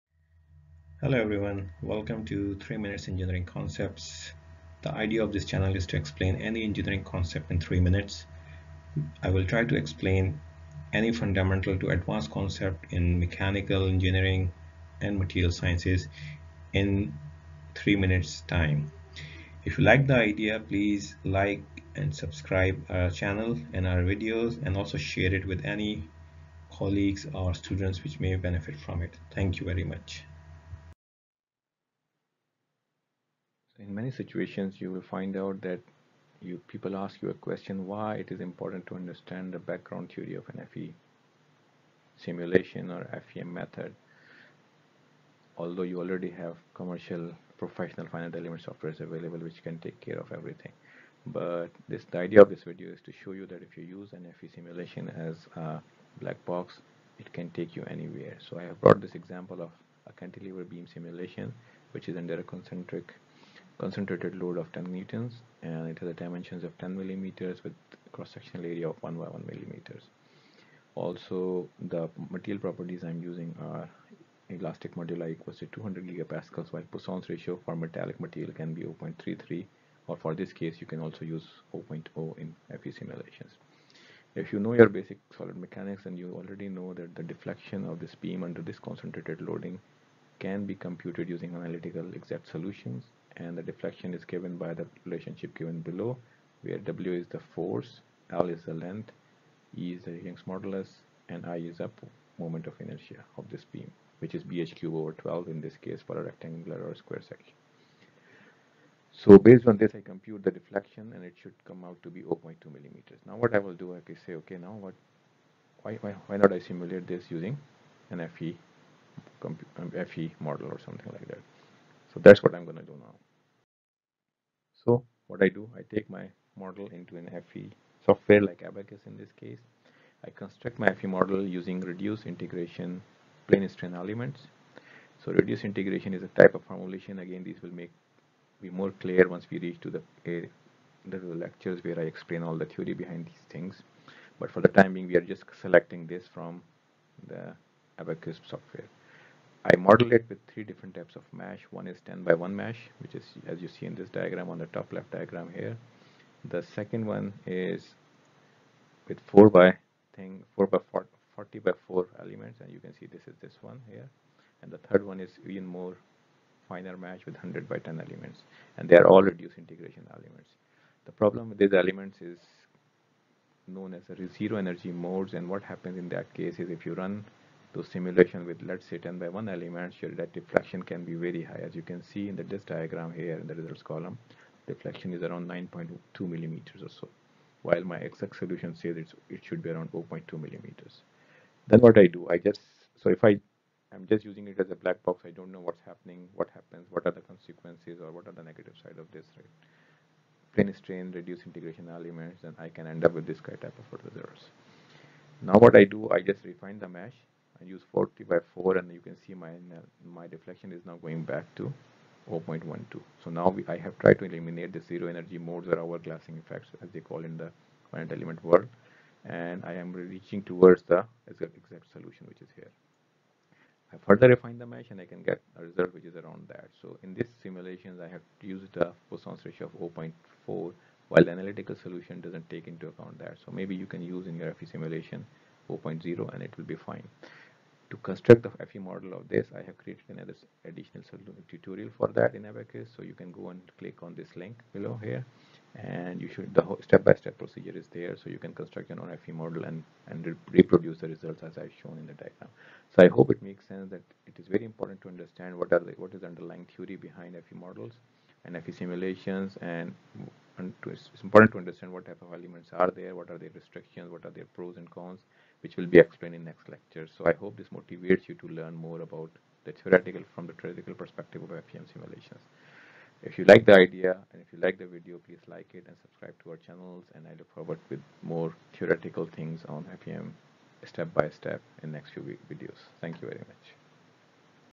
Introduction to Finite Element Methods (FM) - Part 3 - Why to learn FEM theory? Zero Energy modes
This video is part of the FEM lecture series. It explains why it is important not to use FEM as a blackbox and understand the fundamental theory behind FEM. It also discusses the effect of zero energy modes in reduced integration elements and how to eliminate. The FE model used in the video can be constructed using tutorial